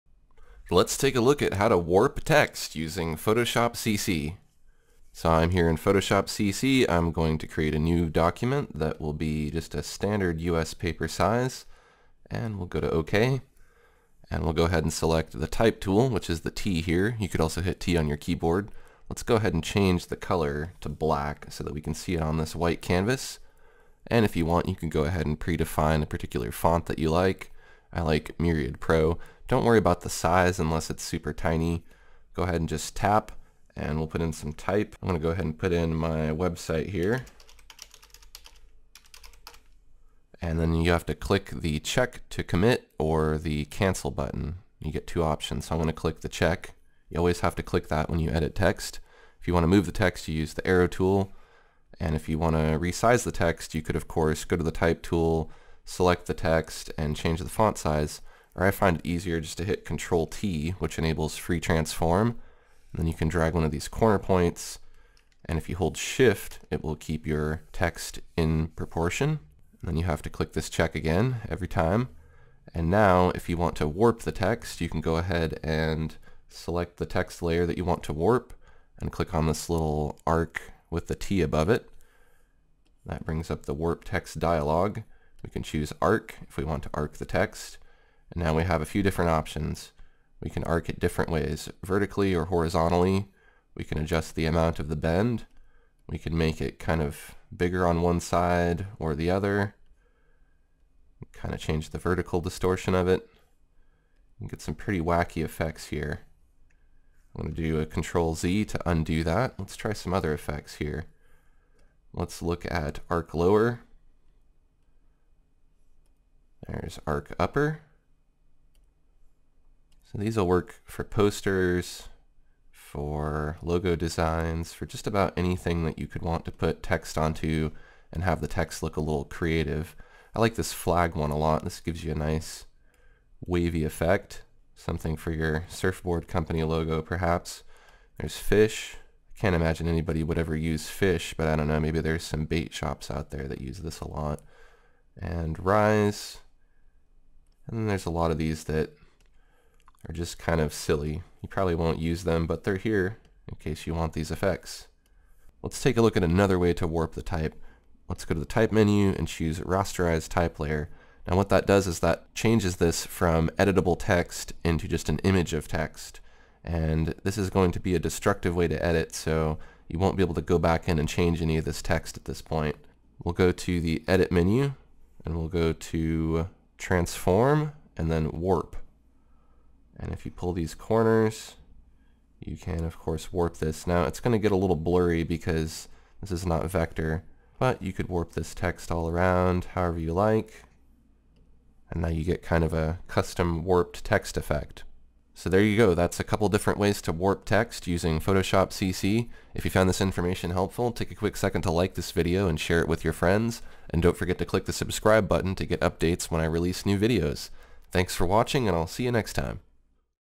How To WARP TEXT with Photoshop
A video tutorial on how to warp and distort text using Adobe Photoshop CC.